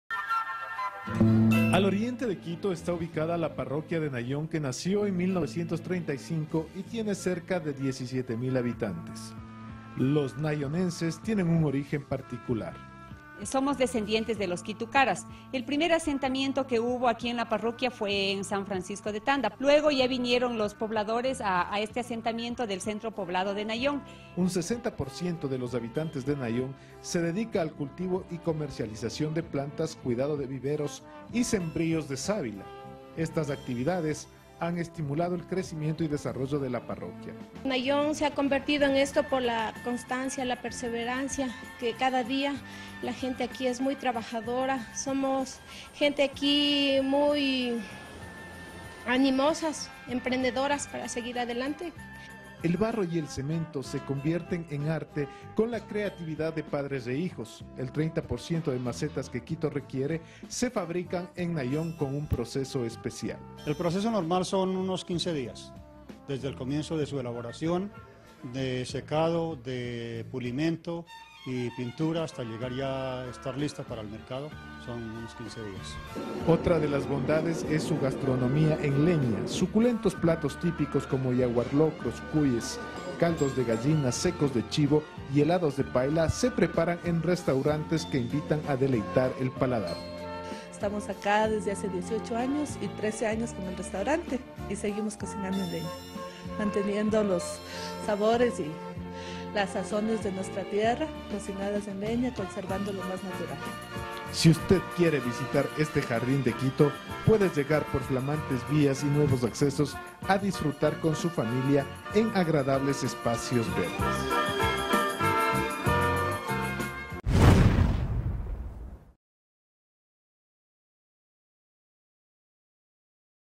Opening Parroquia Nayón cantón Quito
Reseña histórica, cultural y productiva de la parroquia de Nayón.